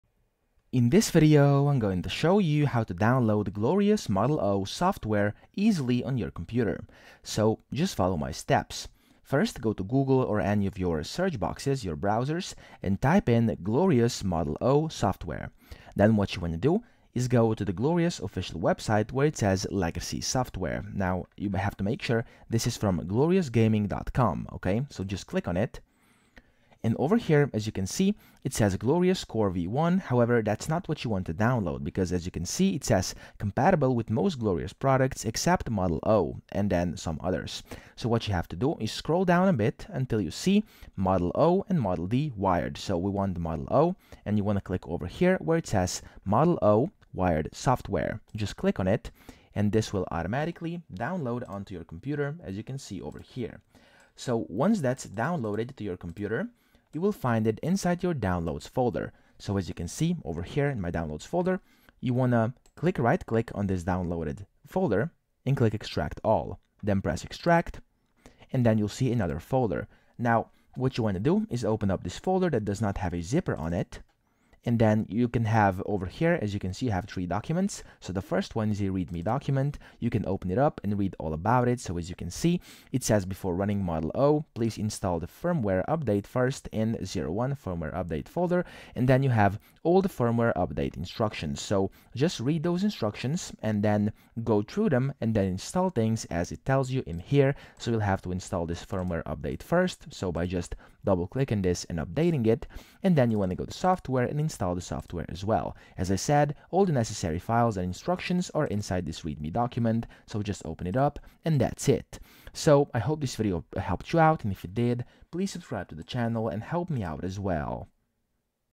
How To DOWNLOAD Glorious Model O Software (FULL GUIDE) 2026
In this video, I will show you how to download Glorious Model O Software on PC/Laptop in 2026. This is a quick tutorial on downloading Glorious Model O Software on your computer.

In case you've been wondering how to download Glorious Model O Software on PC/Laptop, this is the right video for you, since I'll show you how to download and install Glorious Model O Software the easiest way possible.